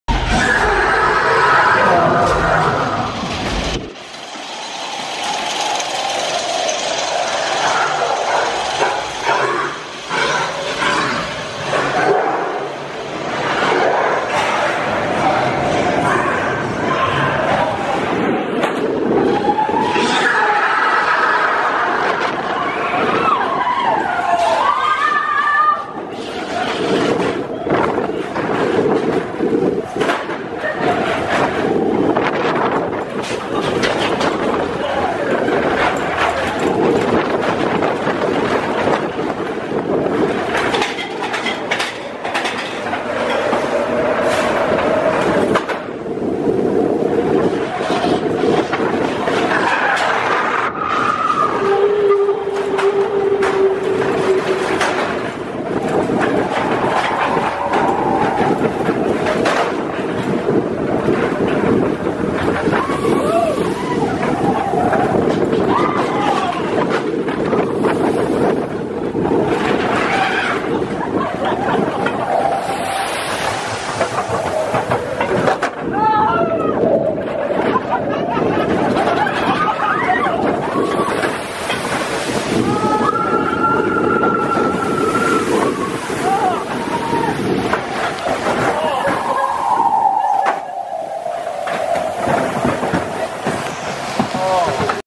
Matterhorn Bobsleds Night POV (Tomorrowland Side), Disneyland Roller Coaster | Non-Copyright
The Matterhorn Bobsleds opened in 1959 as the first tubular steel roller coaster in the world.  This Arrow coaster consists of two separate tracks that weave down the recreation of Switzerland's famous Matterhorn mountain.  Take a ride on this famous coaster!